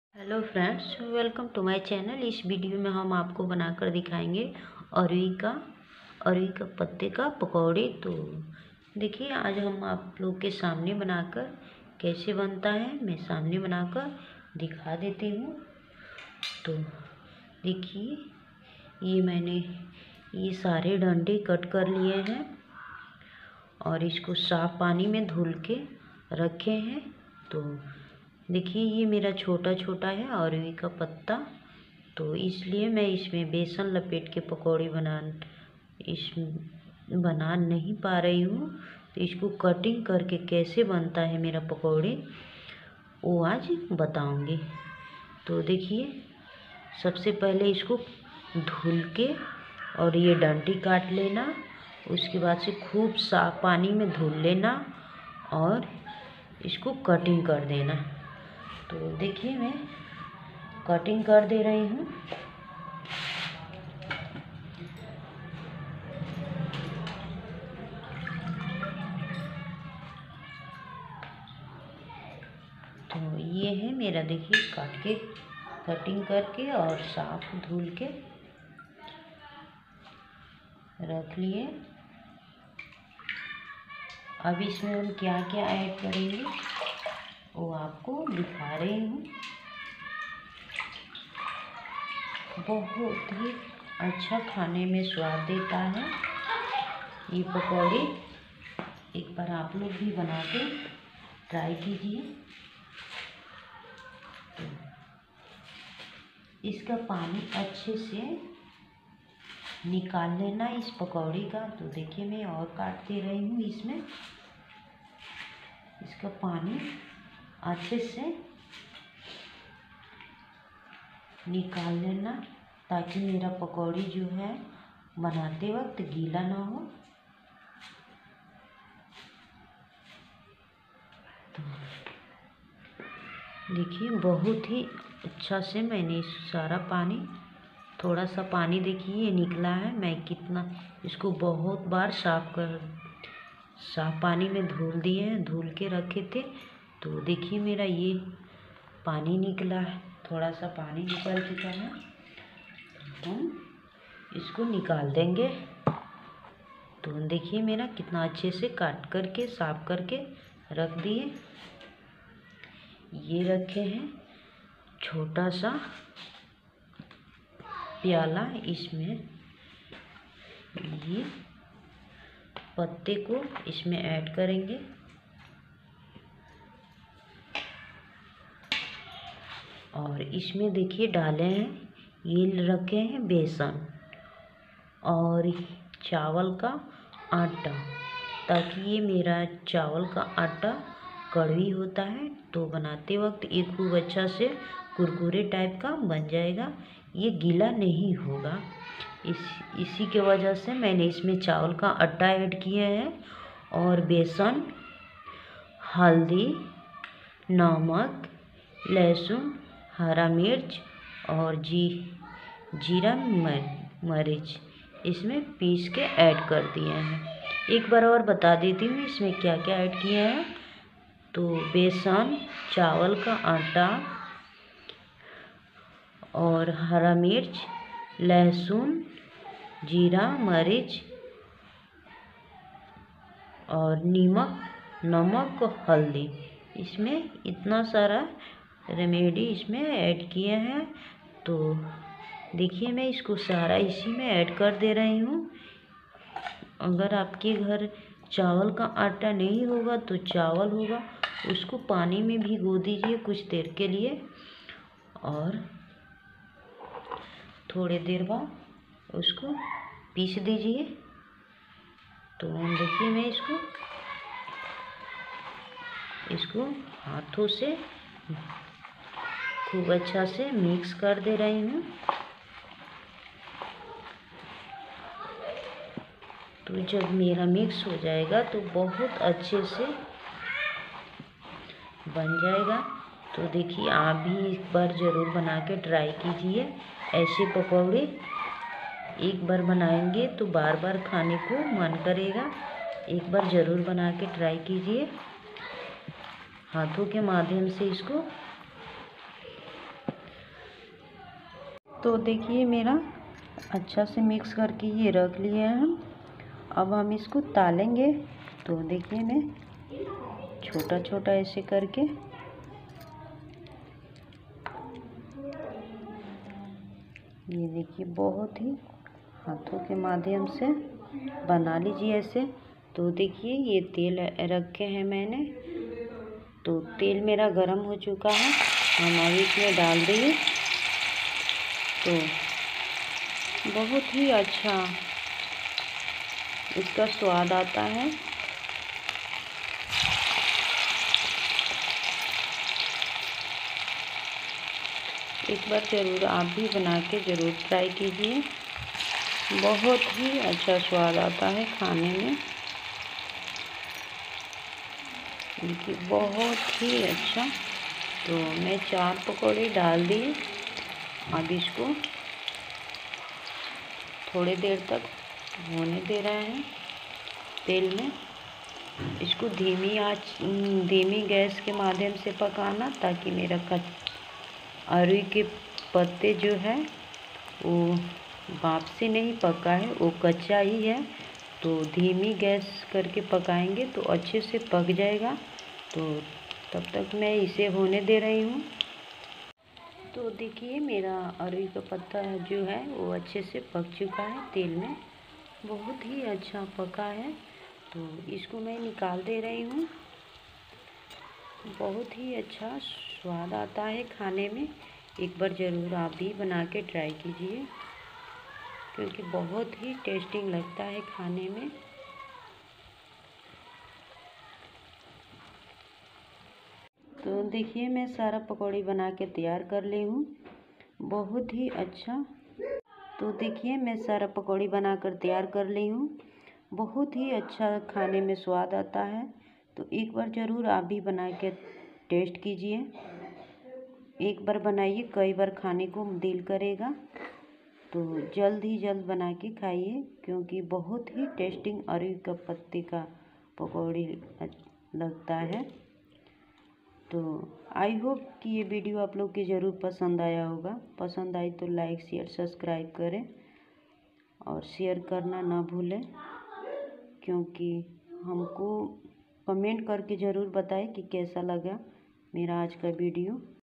अरबी के पत्ते का पकौड़ी कैसे बनाएं
इस वीडियो में हम आपको अरबी के पत्ते का पकौड़ी बनाकर बताए हैं।  1/6/2021